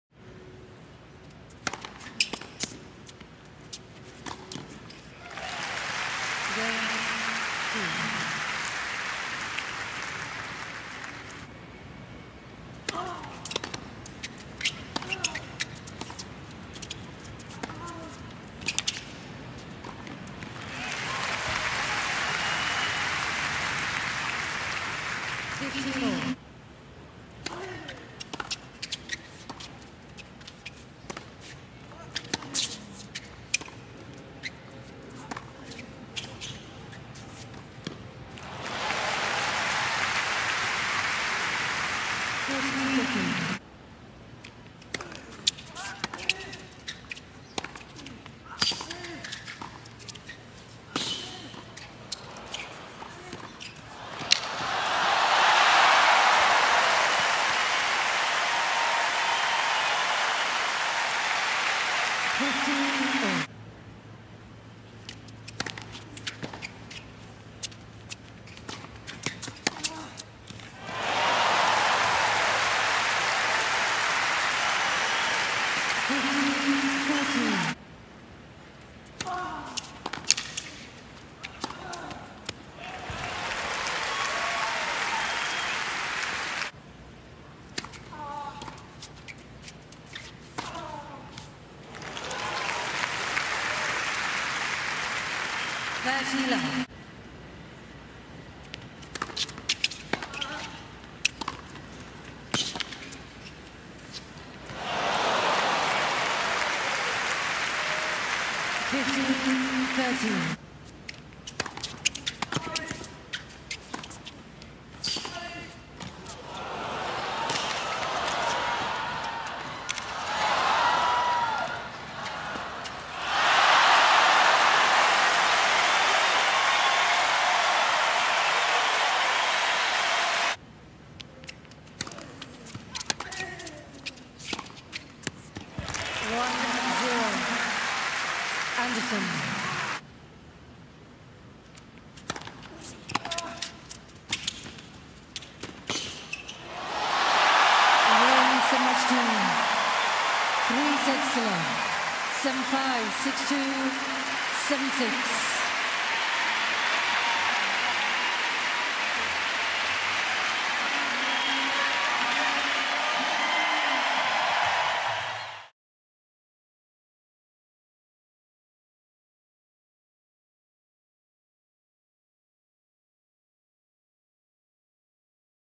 Dominic Thiem Earns a Trip to His First US Open QFs With Win over Kevin Anderson
Highlights from Dominic Thiem's 7-6, 6-2, 7-6 win overKevin Anderson.